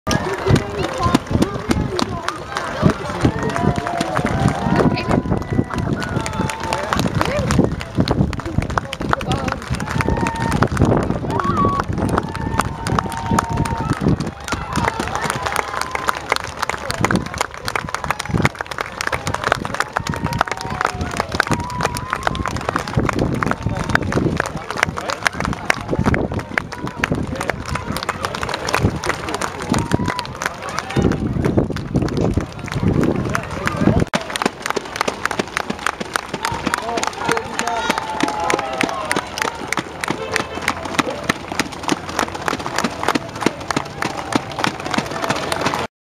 Olympic torch passing through the Isle of Man
2012 Olympic torch starting it's journey in the Isle of Man at the NSC.